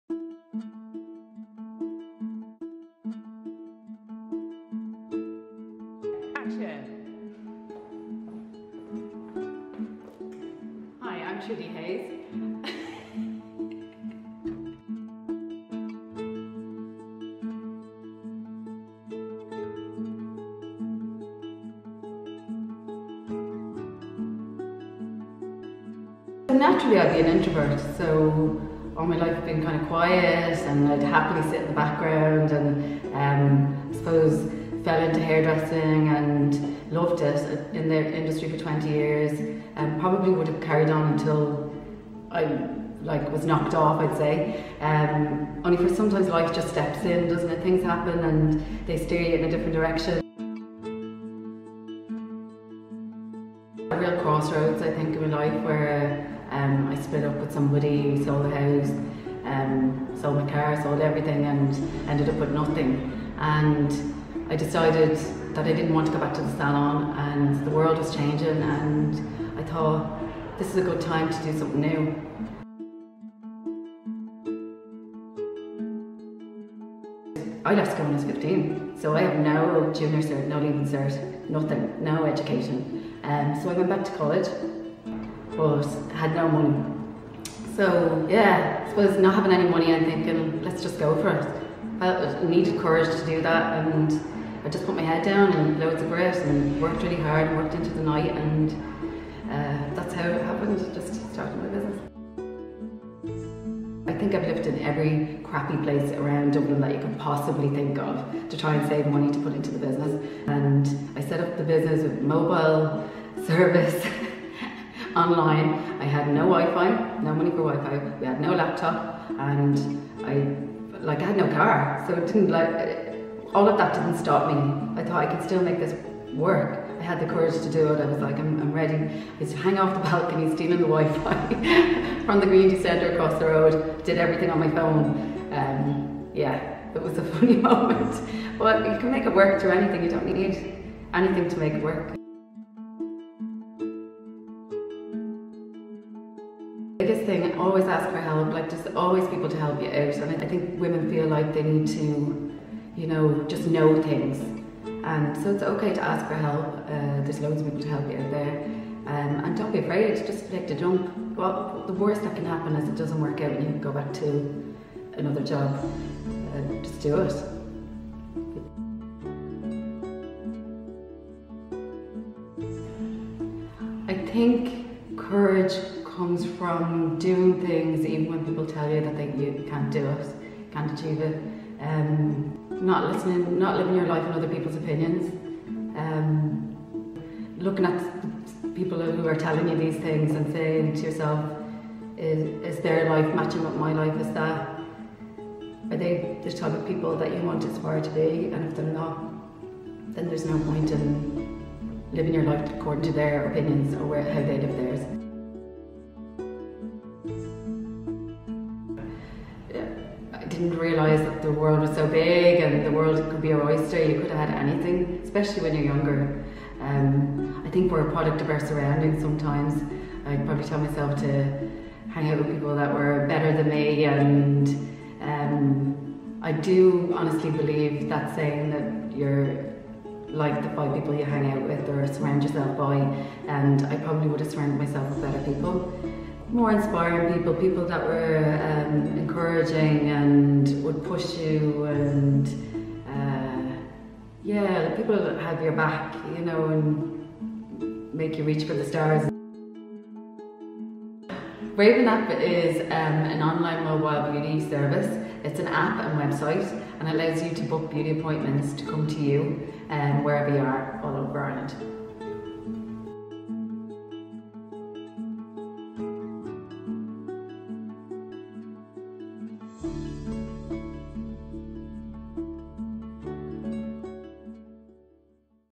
WW Confidence Videos: Trudy Hayes
Woman's Way magazine chats to real women about what gives them the courage and confidence to meet and overcome life's challenges.